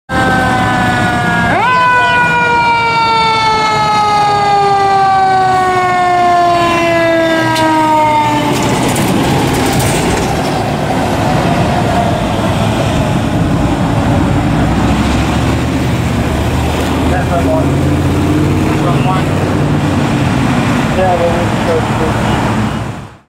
PFD Ladder 8 Responding
Philadelphia Fire Department Ladder 8, covering Ladder Tower 22, responding to and arriving on scene as the RIT on a working building fire on C Street.

Ladder 8 - 2017 KME 100'

Recorded:
 6/2019